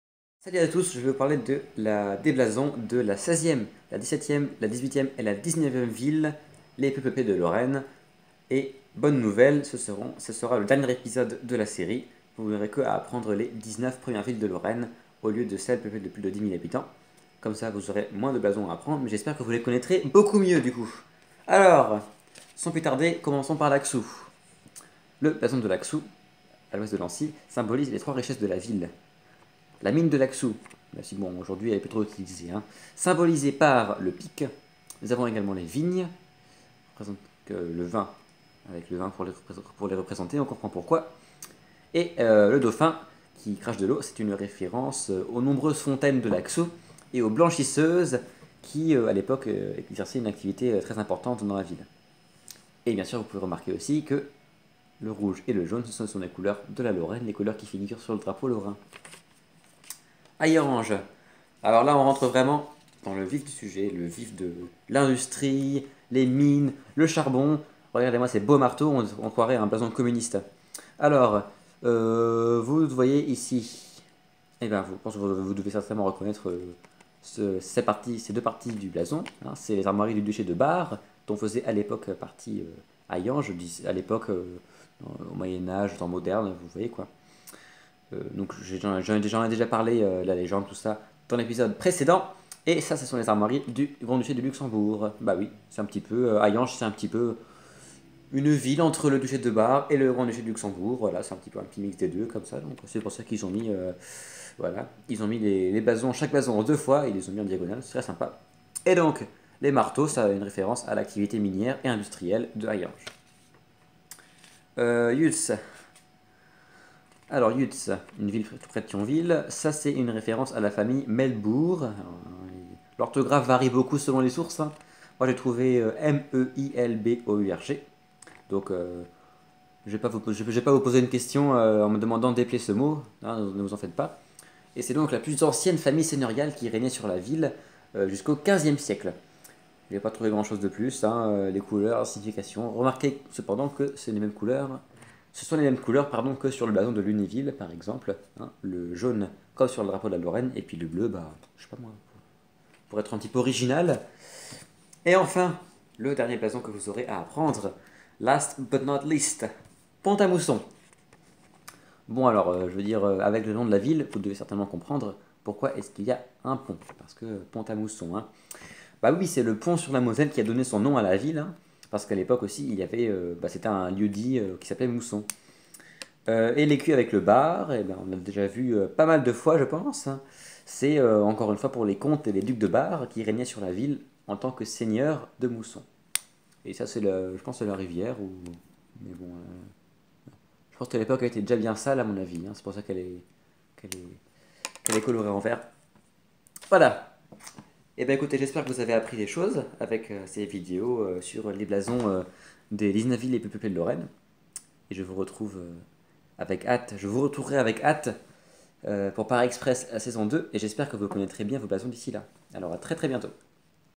Blasons de Lorraine - Partie 5 : Laxou, Hayange, Yutz, Pont-à-Mousson
Dans le cadre de la préparation à Paris Express, la route du Grand Est, les candidats doivent savoir reconnaître et analyser les blasons des 19 villes les plus peuplées de Lorraine. Ici sont expliqués ceux des 16ème, 17ème, 18ème et 19ème villes, à savoir respectivement Laxou, Hayange, Yutz et Pont-à-Mousson.